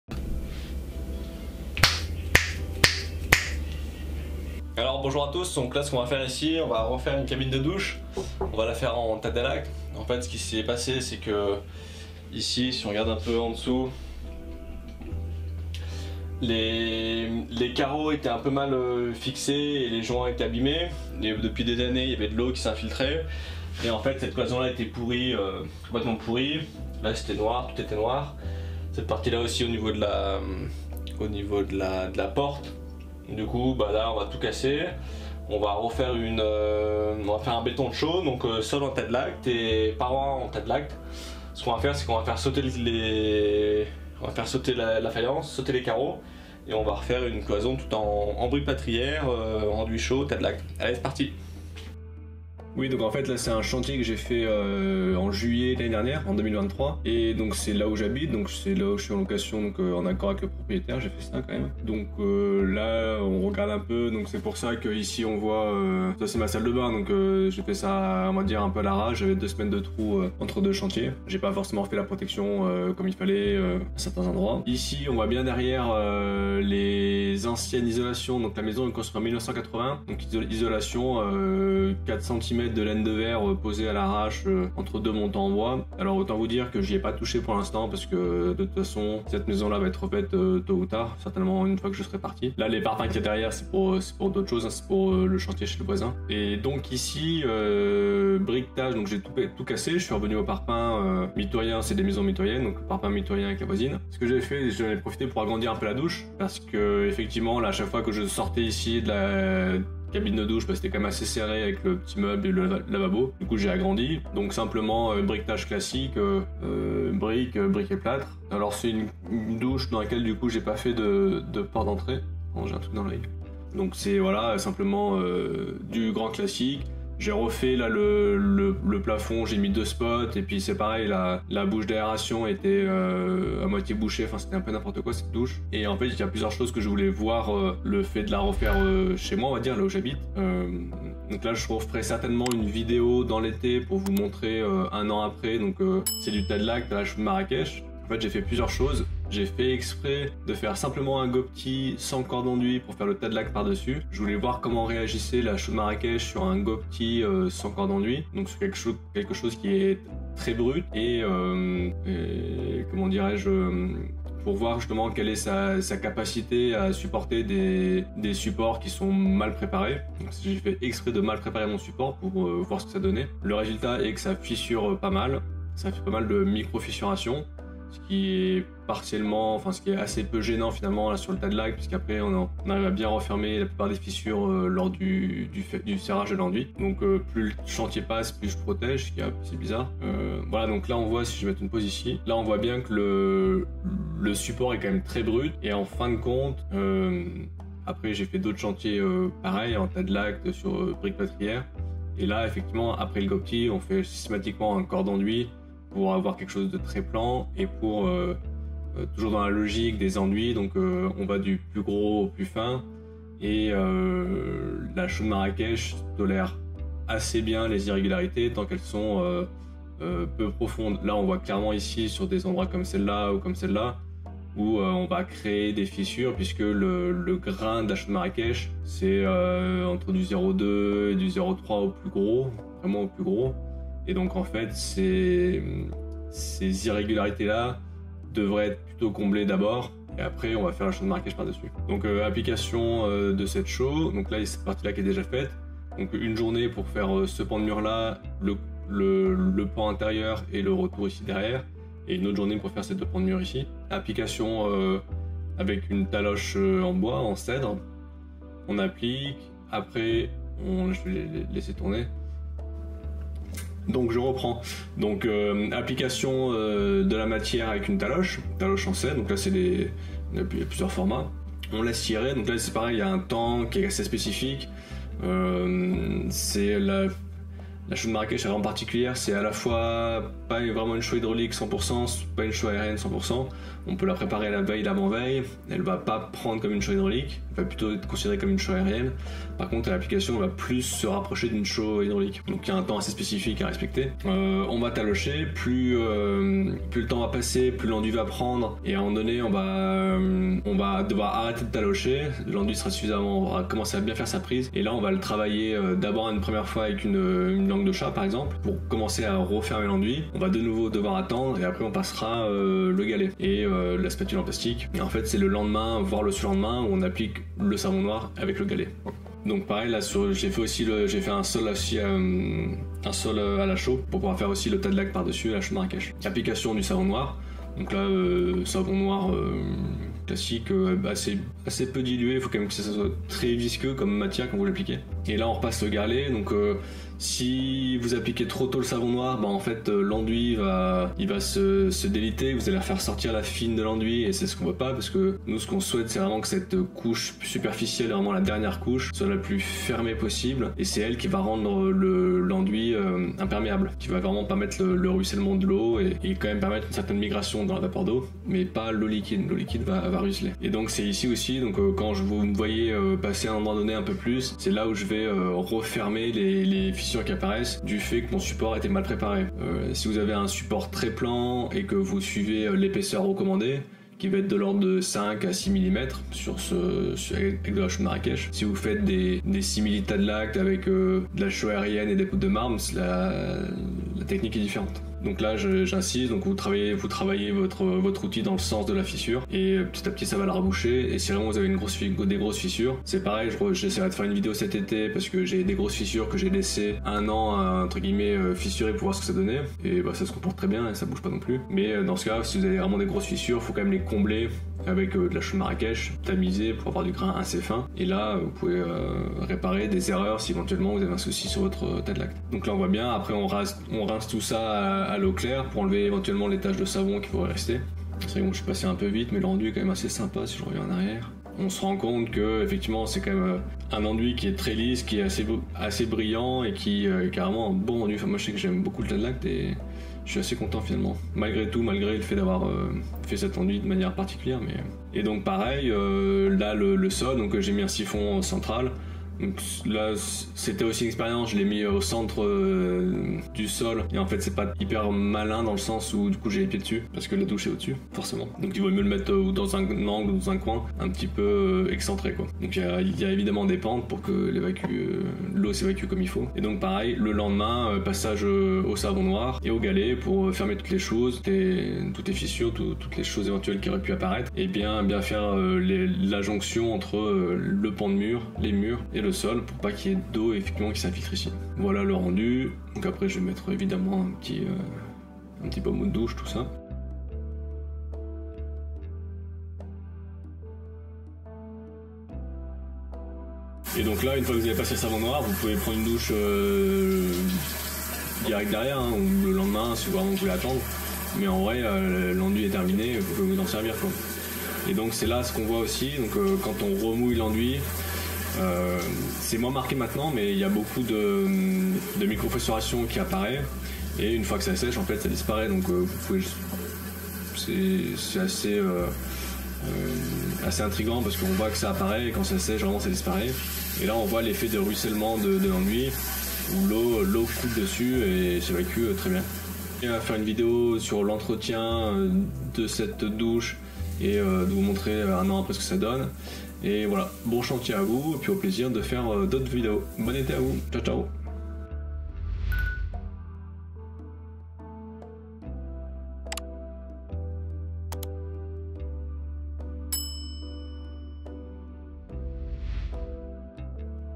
Douche en Tadelakt, murs et sol, , à la chaux de Marrakech. (Timelapse et explications divers)
Création d'une douche, de la démolition de l'ancienne au briquetage, en passant par le gobetis et la réalisation du Tadelakt.
C'est en timelpase photo sur lequel j'apporte des explications au fur et à mesure.
A venir une prochaine vidéo filmée avec plus d'explications.

Bon chantier à vous.